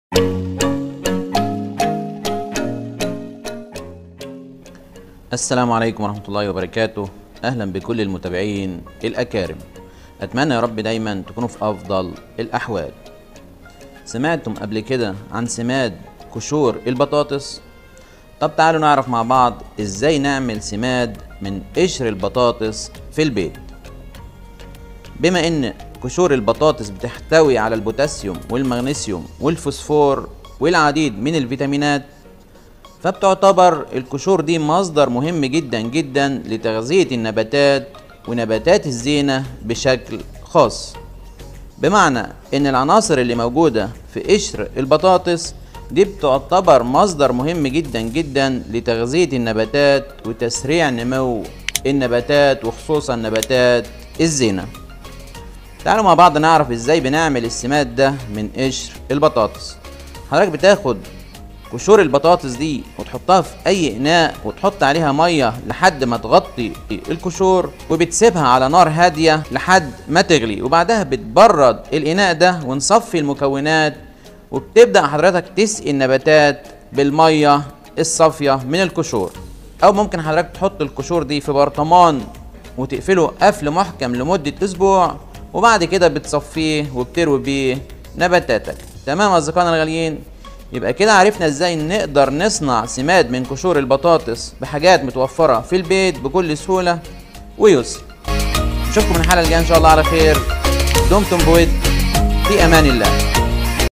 إزاي تعمل سماد للنباتات في البيت من قشور البطاطس؟
في الفيديو هانشوف إزاي تعمل سماد للنباتات في البيت من قشور البطاطس؟
سماد قشور البطاطس